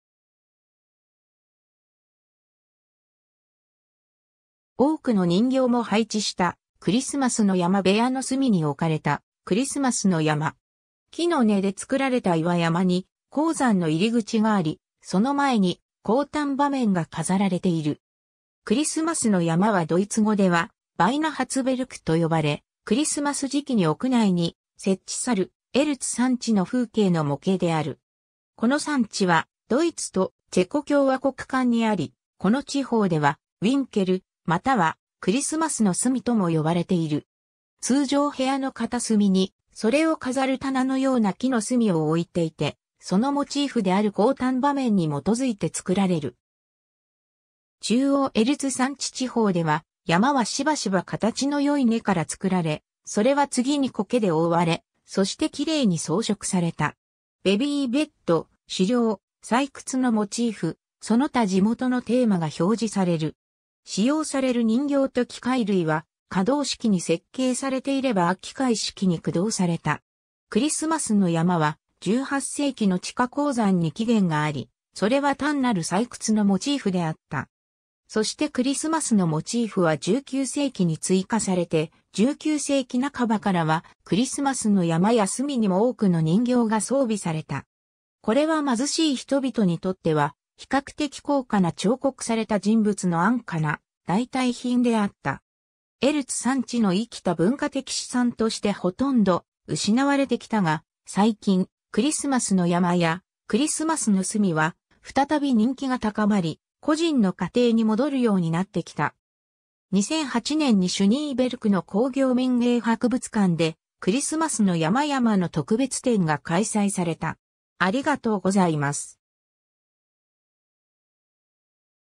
クリスマスの山
多くの人形も配置した「クリスマスの山」 部屋の隅に置かれた「クリスマスの山」。
木の根で作られた岩山に鉱山の入り口があり、その前に「降誕場面」が飾られている。
 クリスマスの山（ドイツ語: Weihnachtsberg）はドイツ語ではヴァイナハツベルクと呼ばれ、クリスマス時期に屋内に設置さるエルツ山地の風景の模型である。
この山地はドイツとチェコ共和国間にあり、この地方では「ウィンケル」または「クリスマスの隅」とも呼ばれている。
 通常部屋の片隅にそれを飾る棚のような木の隅を置いていて、そのモチーフである降誕場面に基づいて作られる。
中央エルツ山地地方では、山はしばしば形の良い根から作られ、それは次に苔で覆われ、そしてきれいに装飾された。
ベビーベッド、狩猟、採掘のモチーフ、その他地元のテーマが表示される。
使用される人形と機械類は、可動式に設計されていれば機械式に駆動された。
 クリスマスの山は、18世紀の地下鉱山に起源があり（鉱山が多いエルツ山地のエルツは、ドイツ語で鉱石の意味）、それは単なる採掘のモチーフであった。
そしてクリスマスのモチーフは19世紀に追加されて、19世紀半ばからは、クリスマスの山や隅にも多くの人形が装備された。
これは貧しい人々にとっては、比較的高価な彫刻された人物の安価な代替品であった。
 エルツ山地の生きた文化的資産としてほとんど失われてきたが、最近「クリスマスの山」や「クリスマスの隅」は再び人気が高まり、個人の家庭に戻るようになってきた。
 2008年にシュニーベルクの鉱業民芸博物館でクリスマスの山々の特別展が開催された。